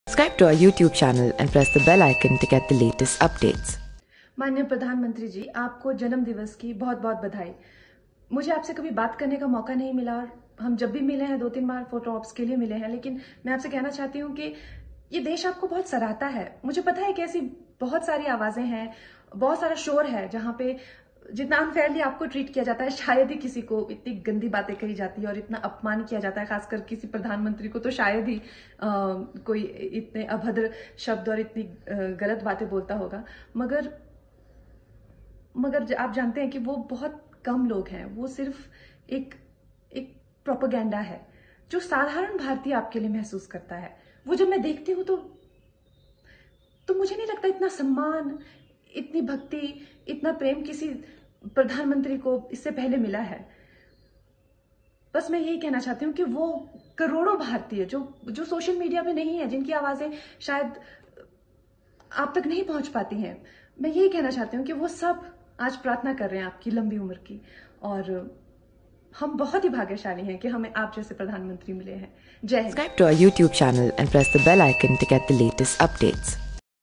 PM Modi के Birthday पर Kangana Ranaut ने शेयर किया शानदार वीडियो, दिया ये संदेश
PMModi के Birthday पर KanganaRanaut ने शेयर किया शानदार वीडियो, दिया ये संदेश